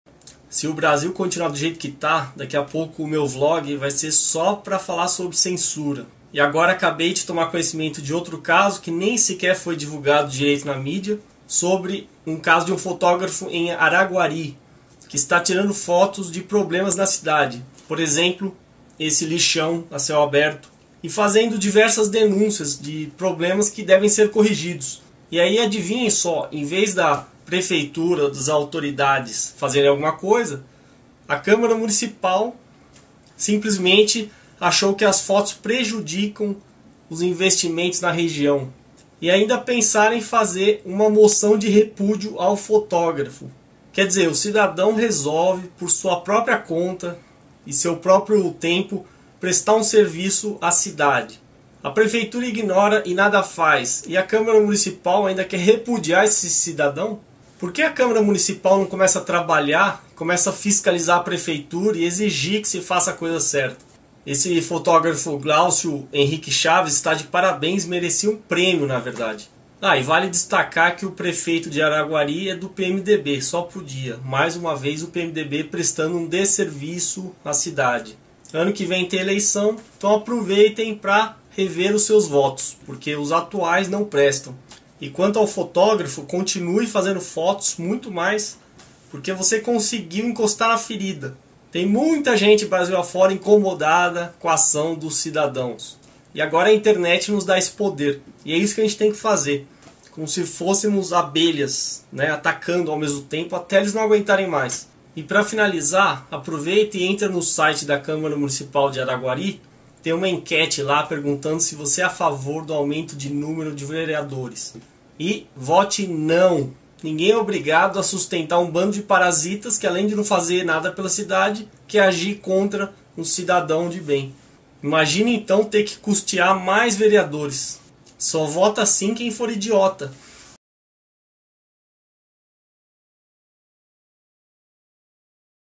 Vereadores de Araguari-MG querem censurar fotógrafo
Saia Justa

Uma comitiva de vereadores que foi a Brasília voltou chateada com um fato que presenciaram. Durante visita, na quarta-feira, eles foram ao MEC - Ministério da Educação - que tem um dispositivo que permite localizar, via satélite, áreas ideais para a construção de creches com recursos federais. Durante as buscas, foram mostradas várias fotos postadas pelo fotógrafo araguarino que assina seus trabalhos como Glaucio Henrique Chaves e que estariam prejudicando em muito a imagem de Araguari por todo país.

Saia Justa II

O fotógrafo seria responsável pela maioria das fotos do município postadas no programa de satélite do Google, de grande acesso mundial. O fotógrafo teria registrado imagens de 2005 e, no satélite, podem ser acessadas através de janelas, cujas fotografias ele legendou com extrema crítica, não escapando sequer uma carroça de entulho flagrada em lotes abandonados. Constrangido com os vereadores, o operador do sistema sugeriu que alguma medida fosse tomada para retirar as legendas pejorativas em relação à cidade de Araguari.

Estrago

Para os vereadores, grandes empresários de toda parte do país usam essa ferramenta para identificar áreas em cidades onde existe o interesse de investir e, neste caso, Araguari, certamente, pode ter sofrido perda de oportunidades de negócios, por causa da atitude do fotógrafo. Certamente, o fato é desagradável, mas não se pode ignorar que a cidade precisa mesmo de cuidar melhor de sua imagem. O diálogo com o fotógrafo deve ser aberto, de imediato.

Repúdio

Consta ainda que na próxima terça-feira, quando acontecerá nova sessão da Câmara, os vereadores já preparam uma "Moção de Repúdio" contra o fotógrafo, pois consideram que ele pode não ter do que representam os seus atos. Que tal convidar o rapaz para ser ouvido na casa de leis? O diálogo pode resolver.

Vereadores querem censurar minhas fotos...

"Prova inconteste demonstra que Marcos Coelho de Carvalho pagava R$30,00 por cada voto" afirma advogado

Vereadores de Araguari (29/8/2011)

Evaldo Luiz de Almeida (PTC)

Hamilton Tadeu de Lima Júnior (PMDB)

Aladino Costa (sem informação)

Werley Ferreira de Macedo (PDT)

Wesley Marcos Lucas de Mendonça (PPS)